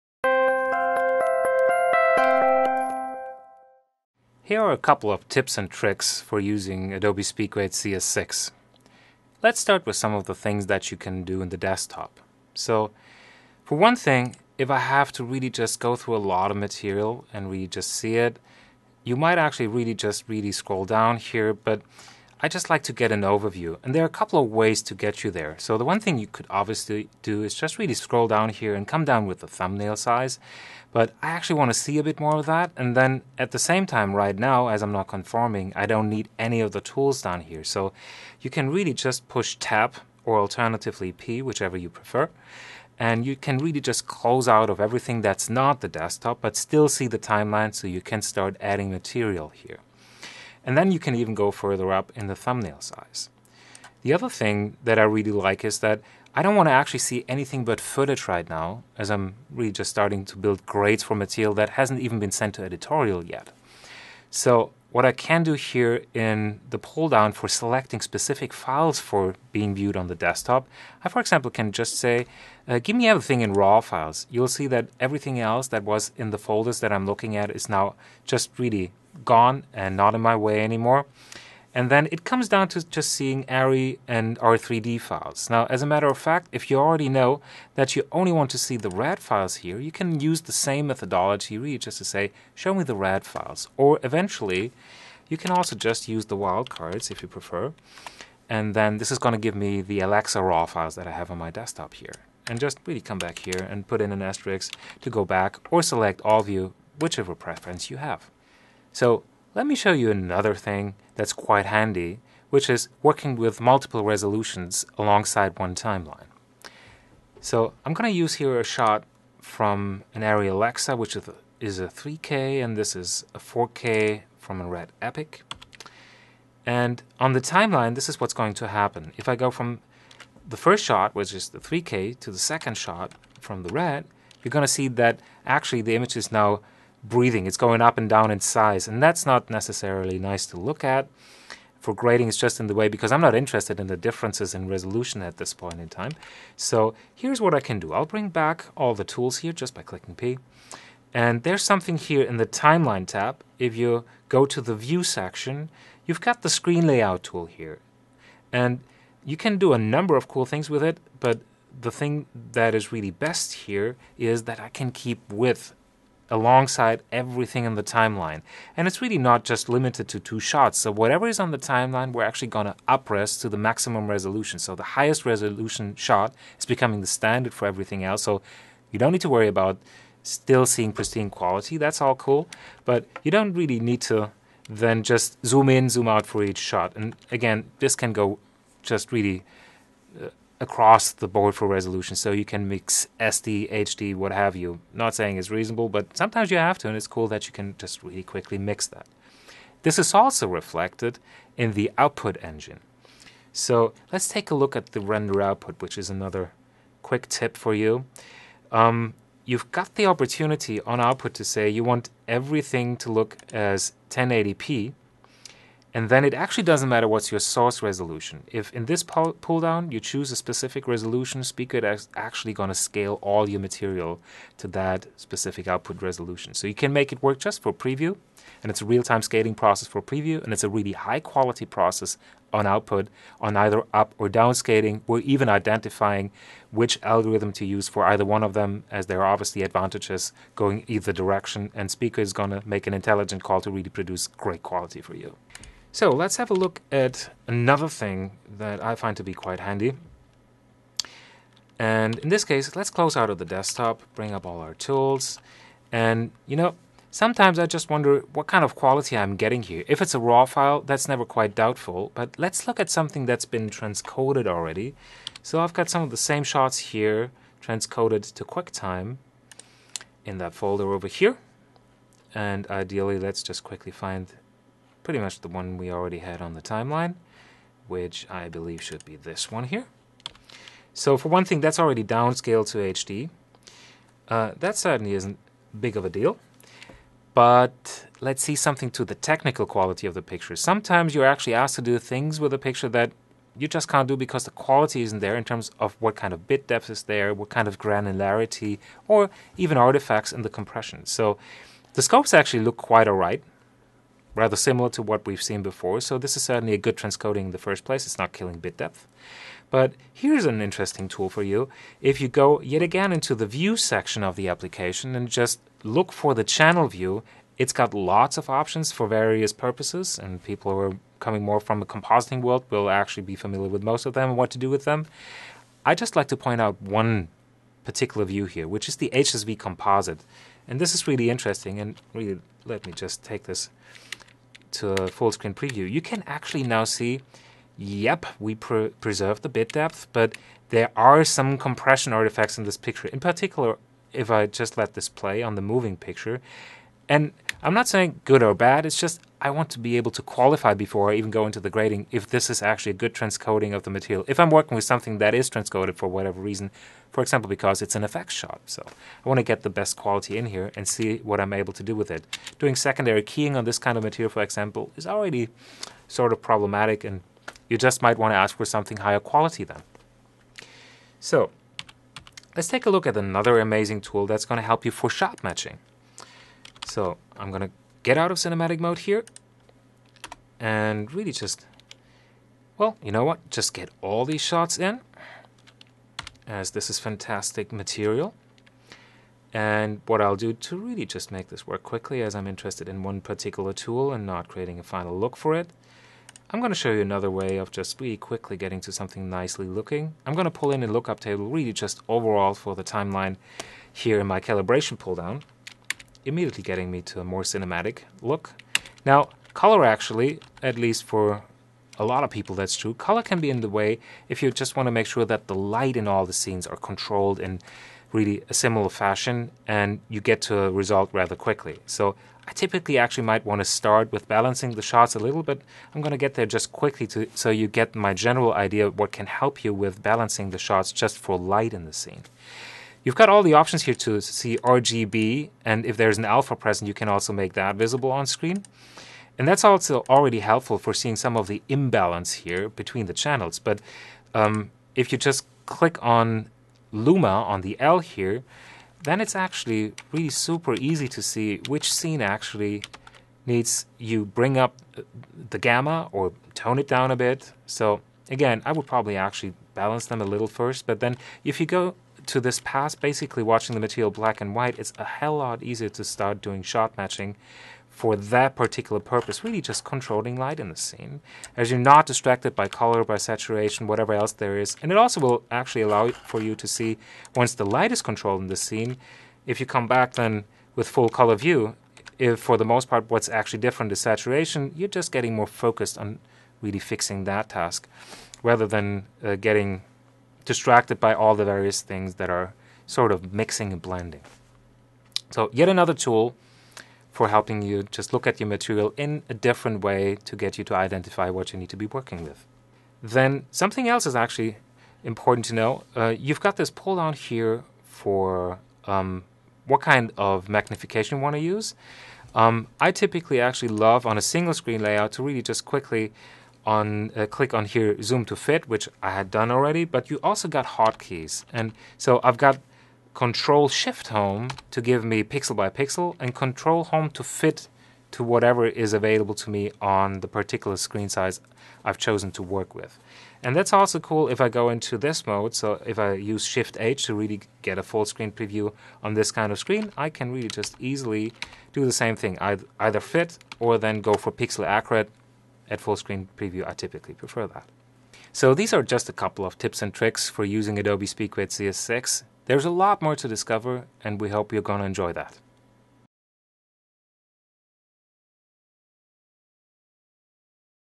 Tips and Tricks for Using SpeedGrade CS6
Sr. Product Manager Patrick Palmer shares his top tips and tricks for using SpeedGrade CS6.